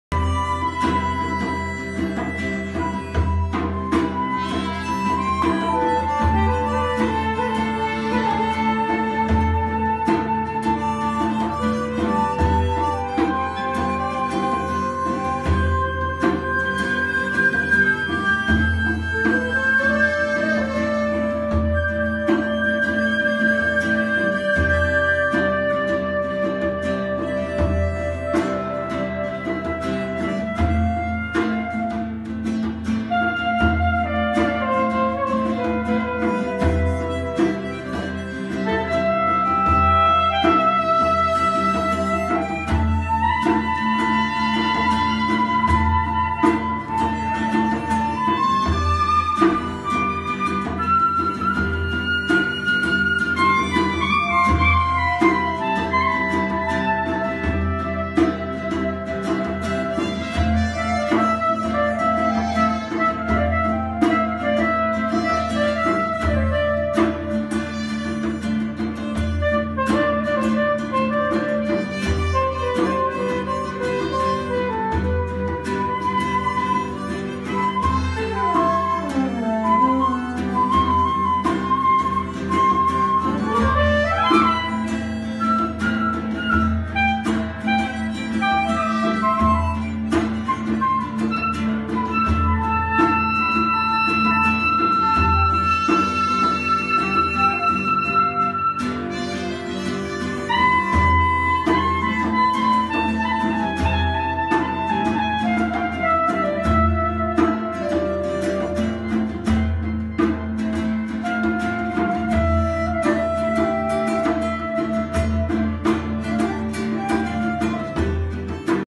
impro Circle jamsessie na de 'pizzaparty' tijdens de weekend workshop 'Modal jazz', mei 2022.
Wil je ook weten wanneer er weer zo'n workshop weekend is?